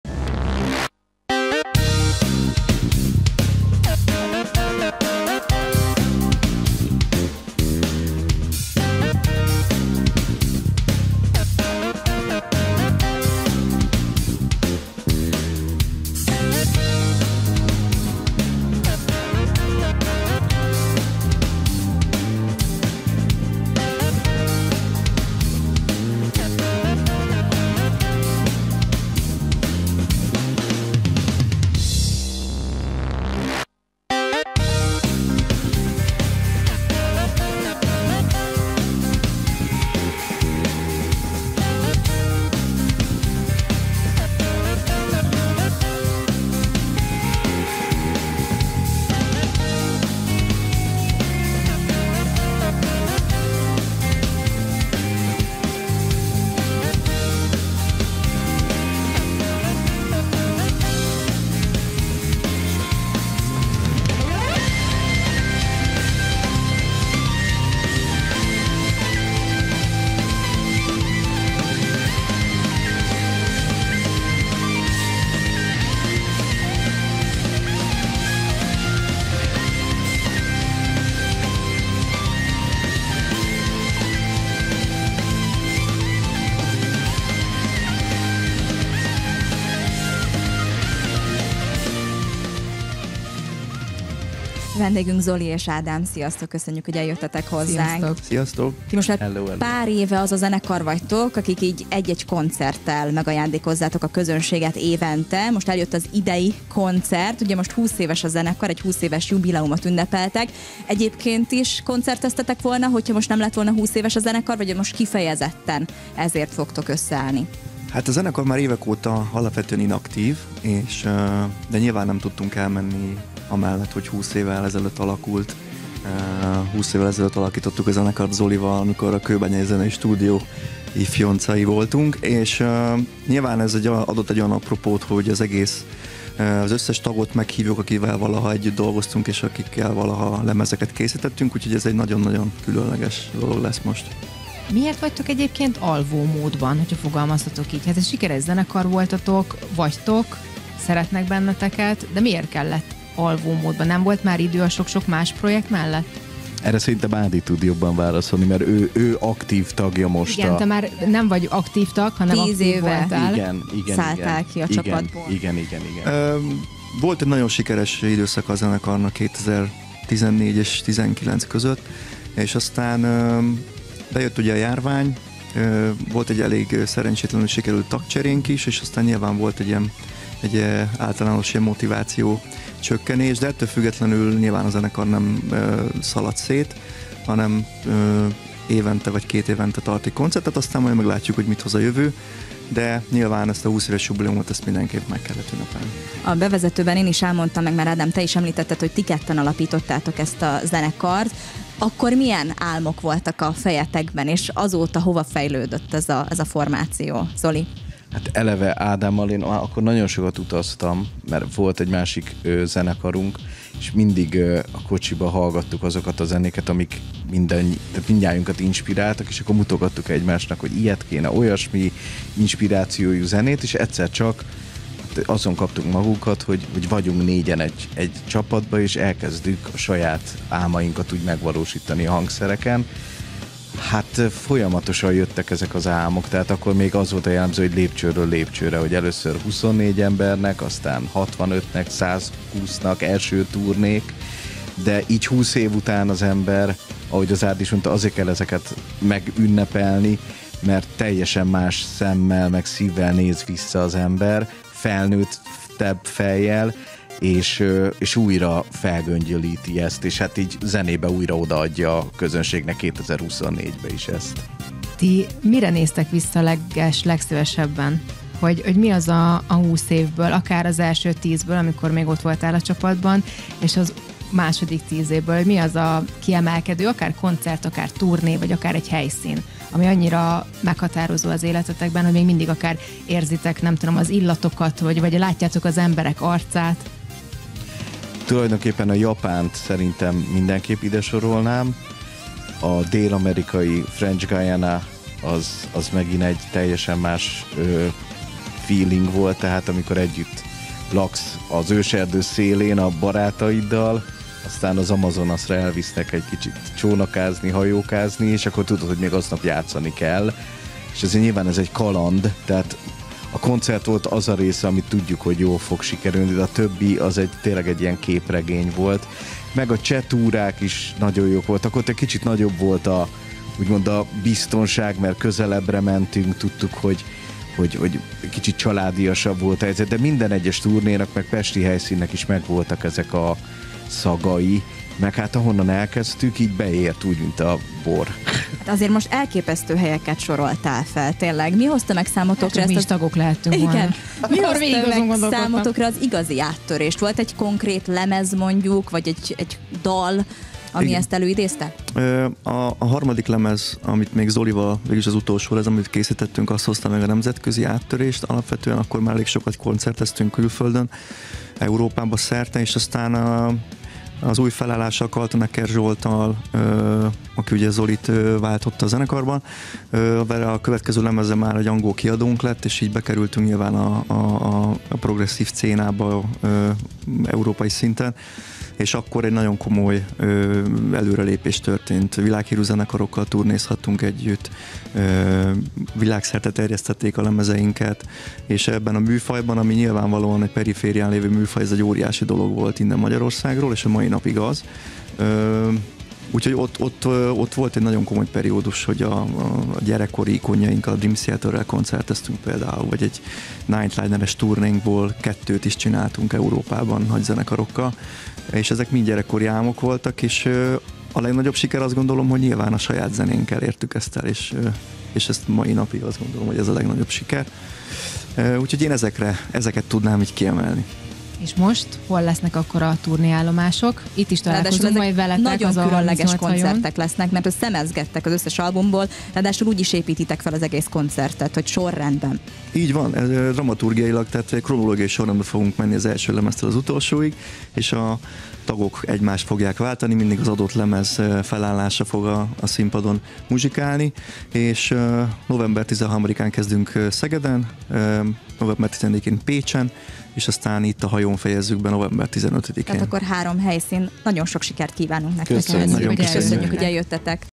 Special Providence
Igazi csemege következik az instrumentális és a progresszív zene kedvelőinek, ugyanis 20. jublieuma alkalmából újra turnéra indul a Special Providence zenekar! Az itthon és külföldön is – a műfajban – elképesztő sikereket elért csapat két alapítótagjával, Markó Ádámmal és Cséry Zoltánnal beszélgettünk.